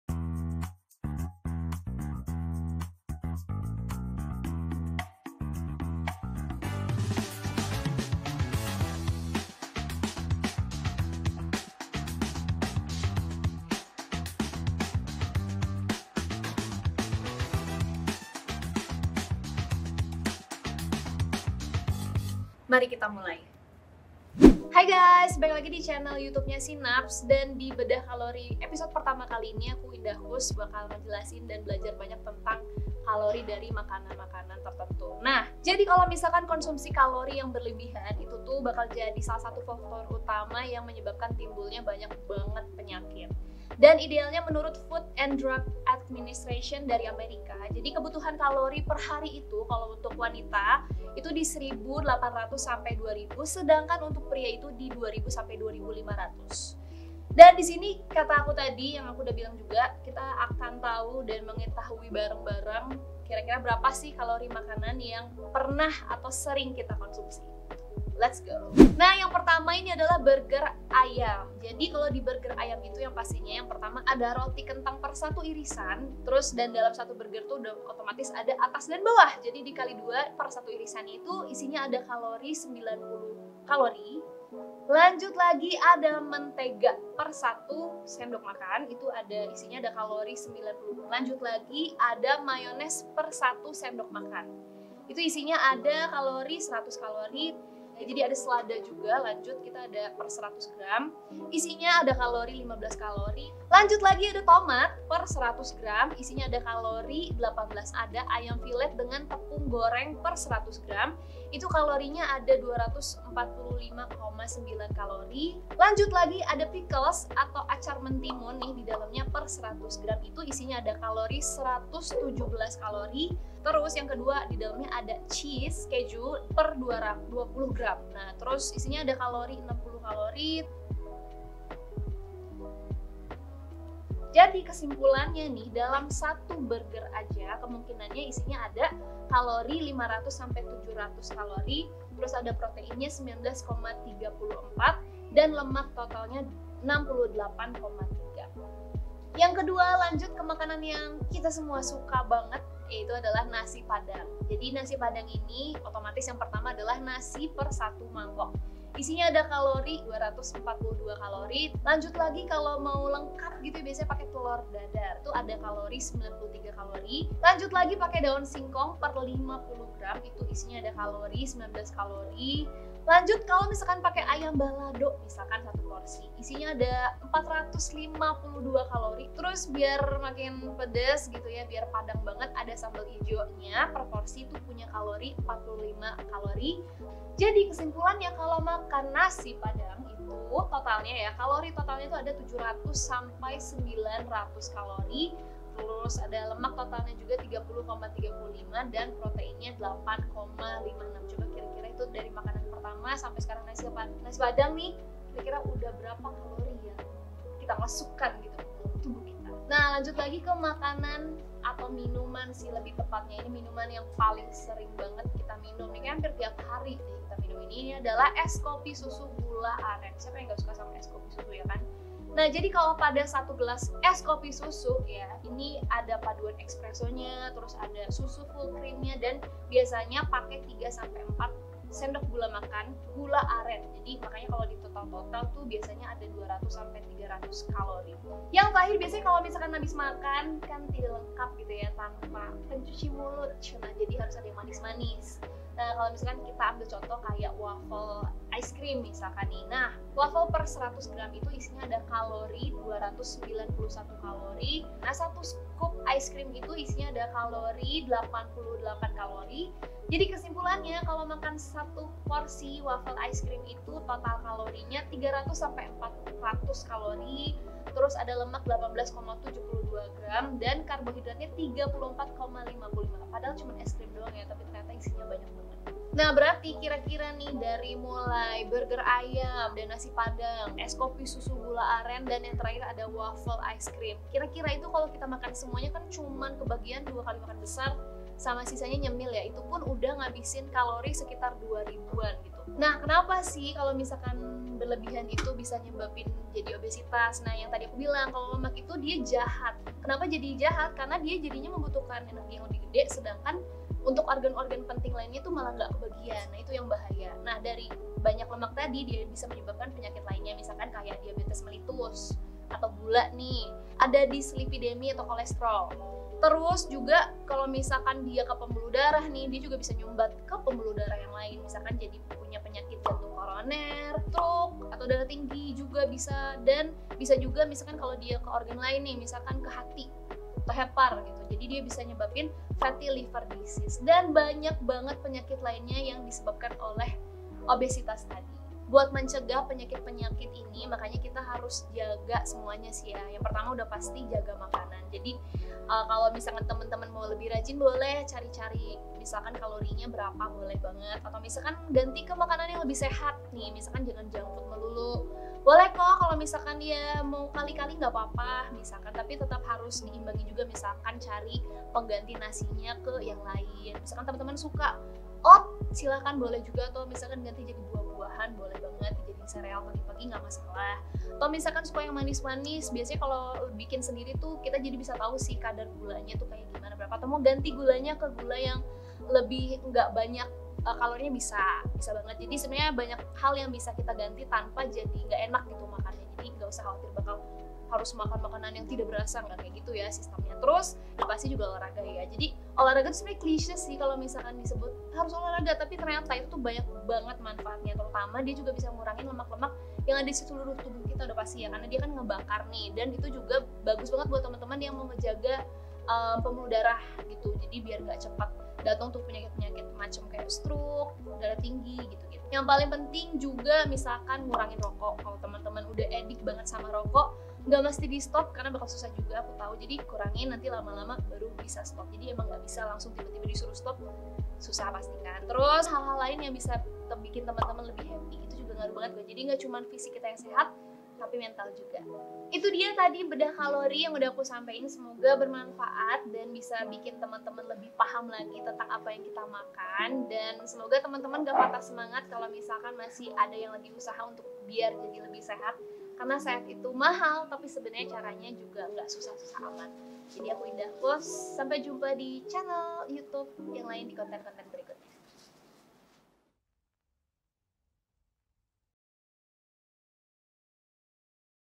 SINAPS "Bedah Kalori" #1 Bersama dr.indahkus
Halo, Kawan SINAPS! 👋👋
Ada sesuatu yang baru nih dari Sinaps!
“BEDAH KALORI”🍽

Disini kita bakal bedah bersama berapa sih kalori yang ada di makanan yang familiar banget tentunya di antara kawan SINAPS agar tidak berlebih dan mencegah risiko penyakit-penyakit akibat kalori yang terlalu banyak pada konsumsi sehari-hari.

Bersama
👩‍⚕️ dr. Indah Kus

Duh, kira-kira berapa ya kalori 🍔 , waffle 🧇 , es kopi ☕️, apalagi…. nasi padang 🍛!!
Daripada kelamaan penasaran, yuk kita tonton video “BEDAH KALORI”👀👀👀

Sinaps Medical Education
Get to know us!

Sampai bertemu di sesi selanjutnya kawan SINAPS! 💫